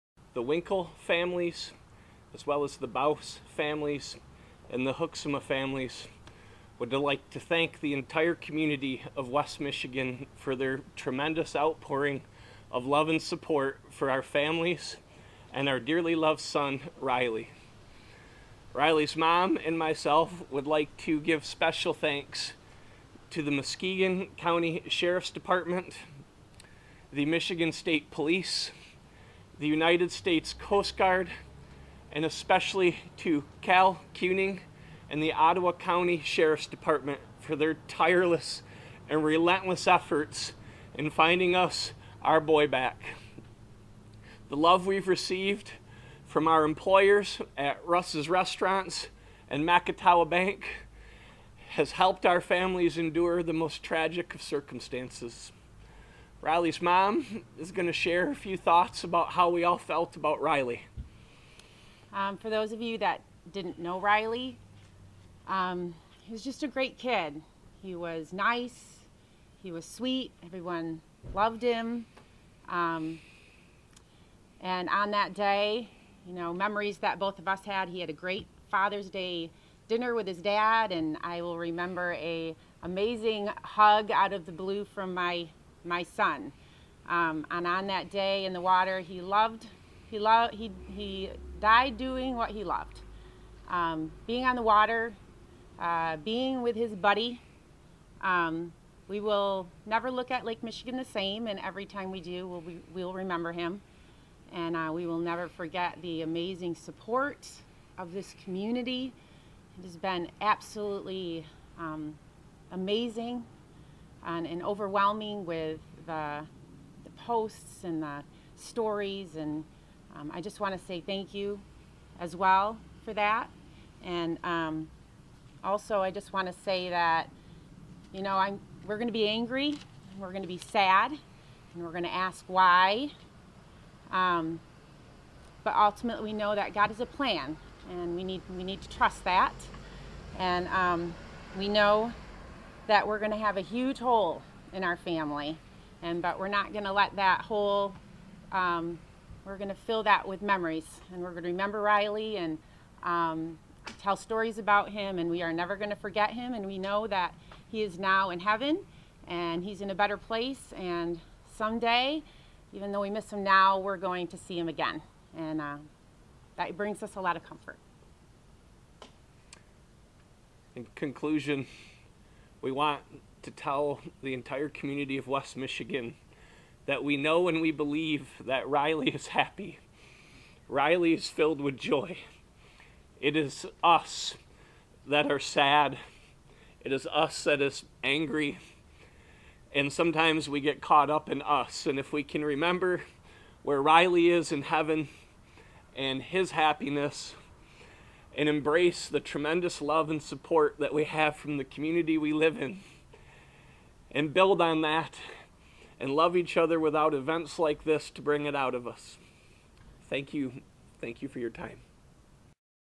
Matt and Kelli Hoeksema June 23
Remarks by Kelly (L) and Matt Hoeksema prior to visitation for their son Riley outside of Christ Memorial Church in Holland, Michigan on June 23, 2016.